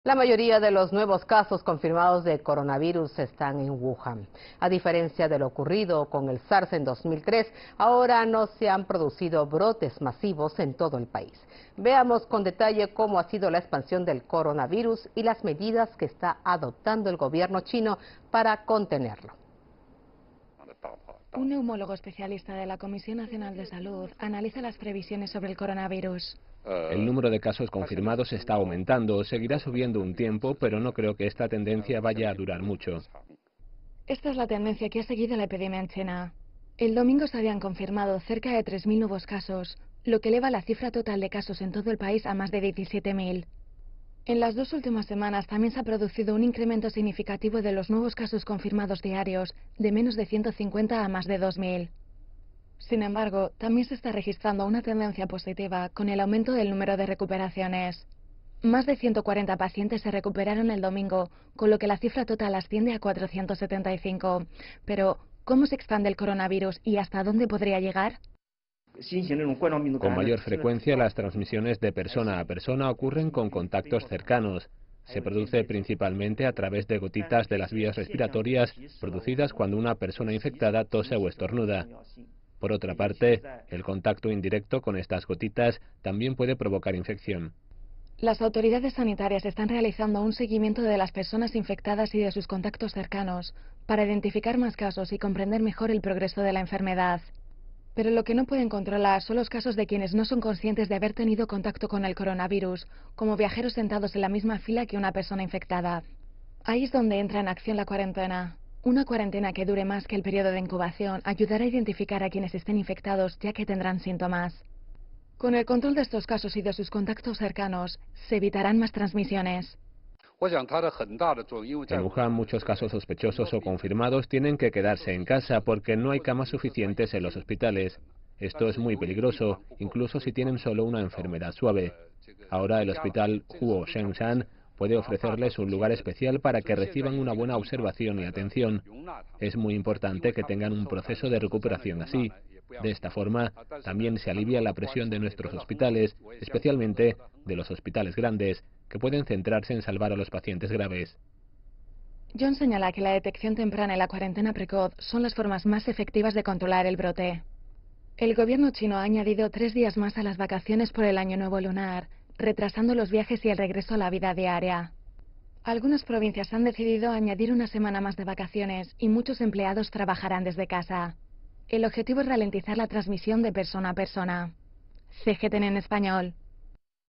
Evolución y medidas de prevención del coronavirus
La mayoría de los nuevos casos confirmados de coronavirus están en Wuhan. A diferencia de lo ocurrido con el SARS en 2003, ahora no se han producido brotes masivos en todo el país. Un neumólogo especialista de la Comisión Nacional de Salud analiza las previsiones sobre el coronavirus.